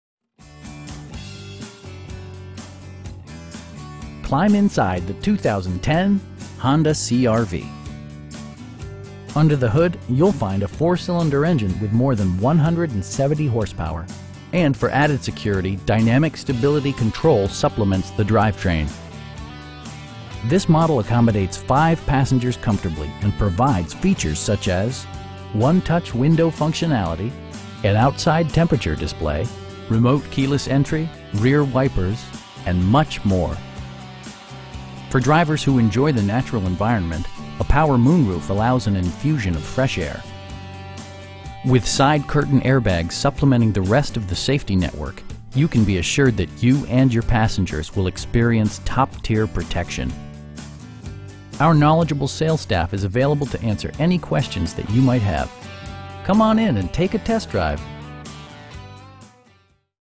2010 Honda CR-V EX in Burlington, VT 05401
ROI Volvo

Climb inside the 2010 Honda CR-V. Under the hood you'll find a 4 cylinder engine with more than 170 horsepower, and for added security, dynamic Stability Control supplements the drivetrain. This model accommodates 5 passengers comfortably, and provides features such as: 1-touch window functionality, an outside temperature display, telescoping steering wheel, power windows, remote keyless entry, rear wipers, and much more. For drivers who enjoy the natural environment, a power moon roof allows an infusion of fresh air. With side curtain airbags supplementing the rest of the safety network, you can be assured that you and your passengers will experience top-tier protection. Our knowledgeable sales staff is available to answer any questions that you might have. Come on in and take a test drive!